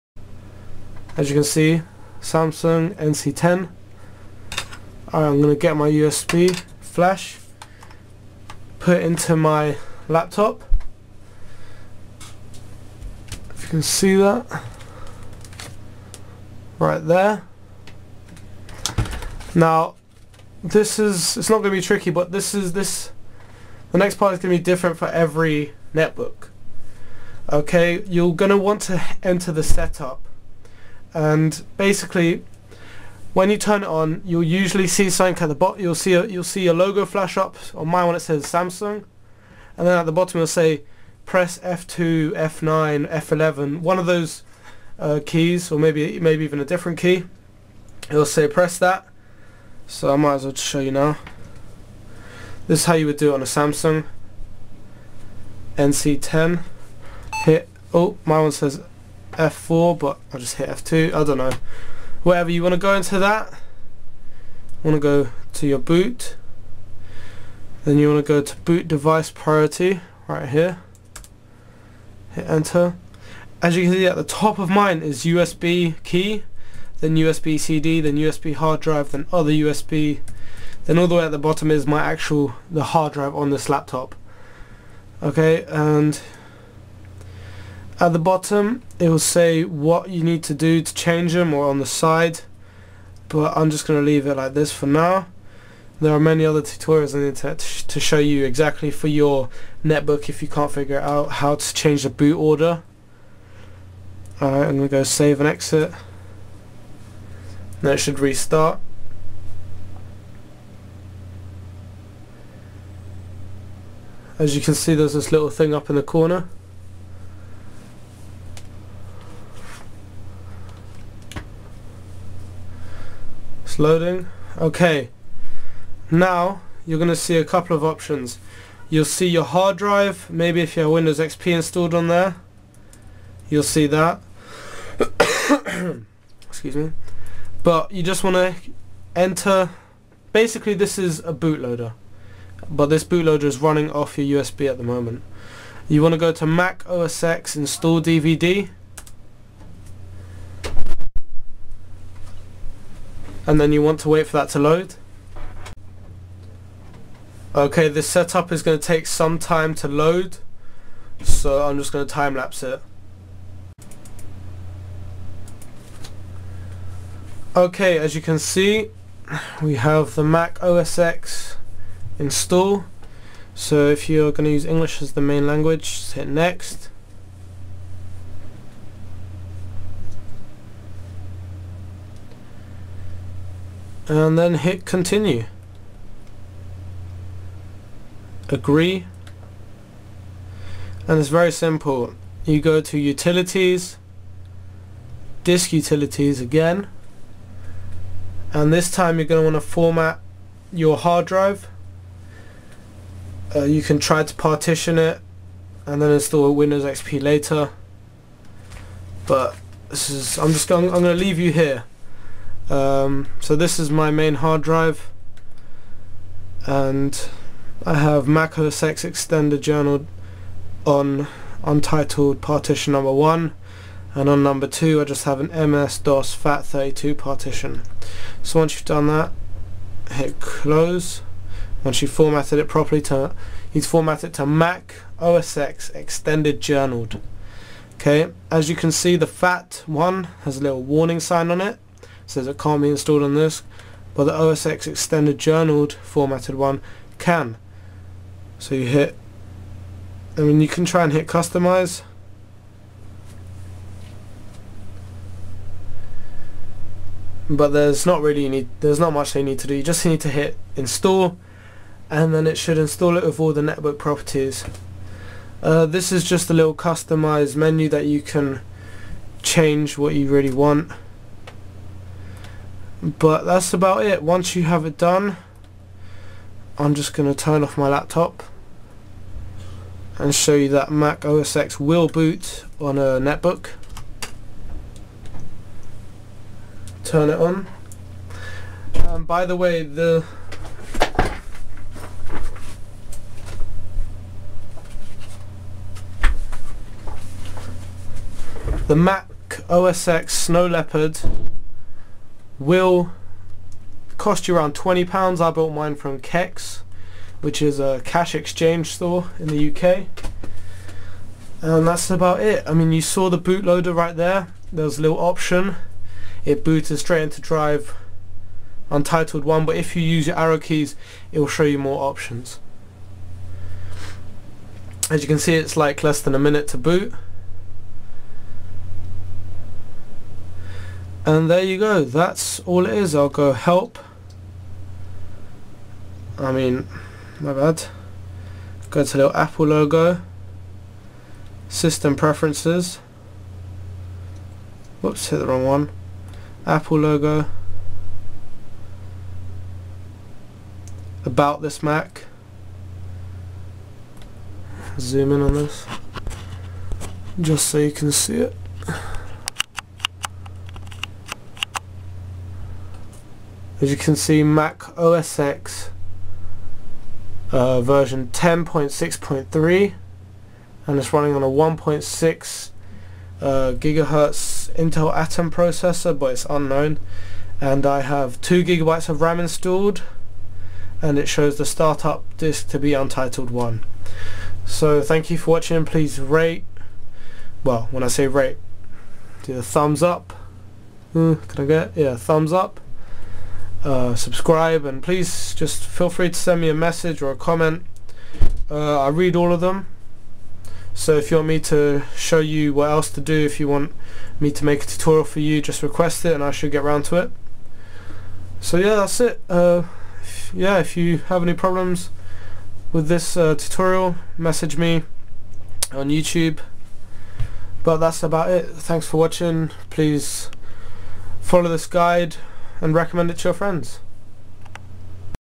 How to install Mac OSX Snow Leopard 10.6.3 on a Netbook [Part 2]
Netbook
Samsung NC10
2GB Ram
1.6ghz Intel Atom Processor